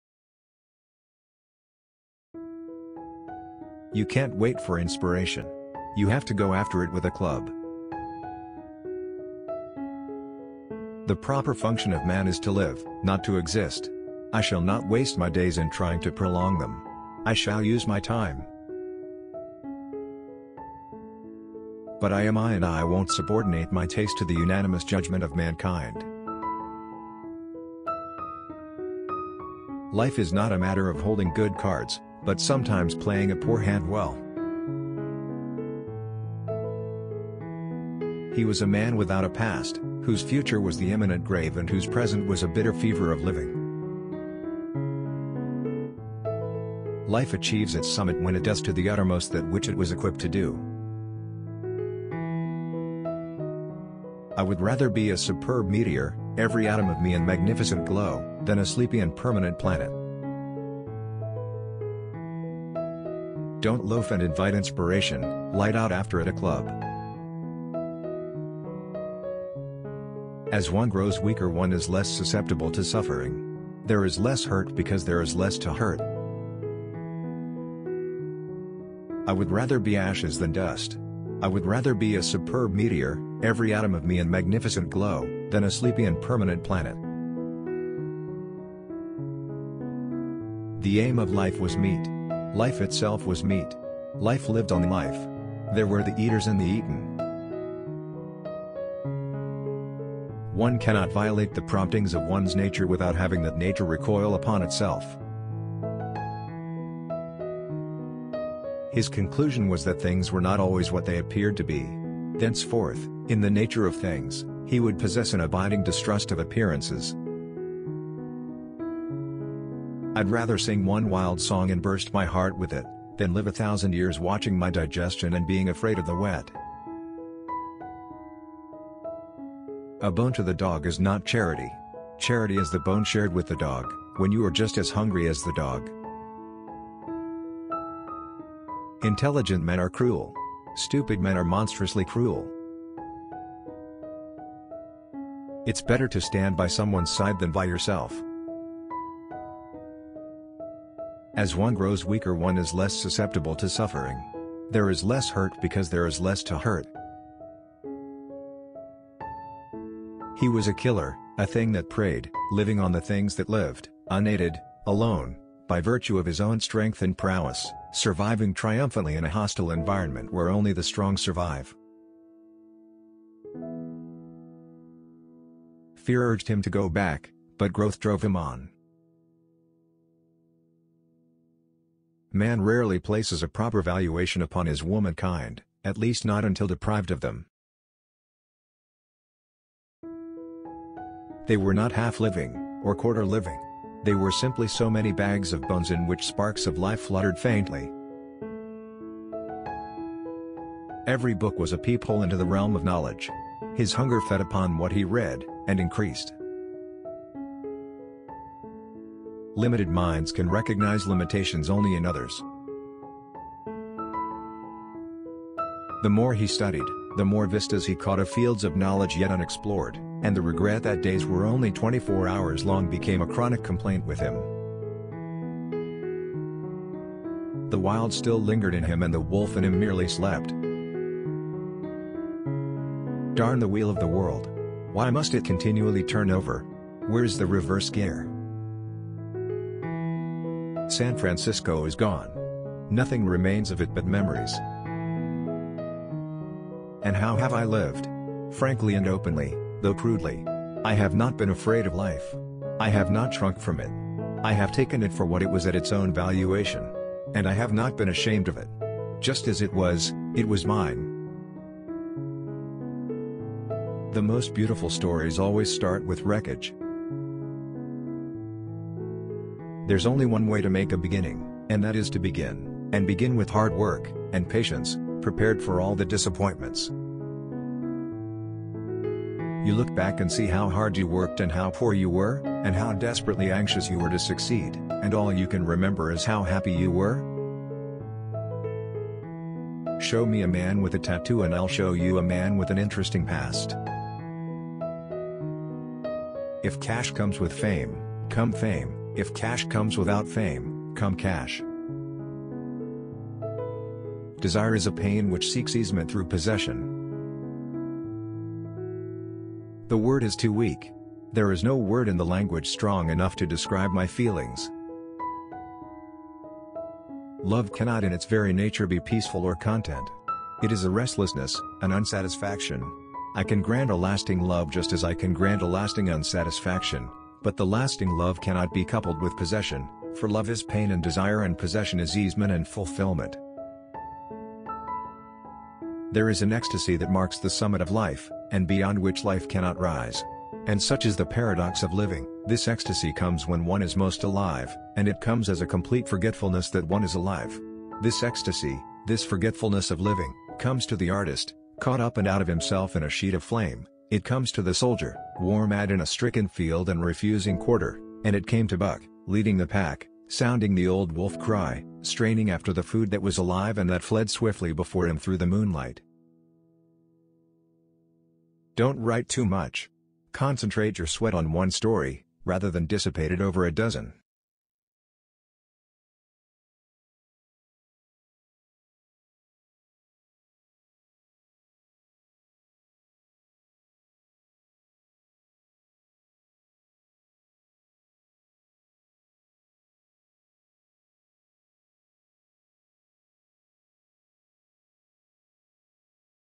Jack London Quotes that Will Change Your Thinking Pattern
John Griffith Chaney London (January 12, 1876 – November 22, 1916) was an American novelist, journalist and social activist. A pioneer of commercial fiction and American magazines, he was one of the first American authors to become an international celebrity and earn a large fortune from writing. He was also an innovator in the genre that would later become known as science fiction.

Famous Quotes From American Writer Jack London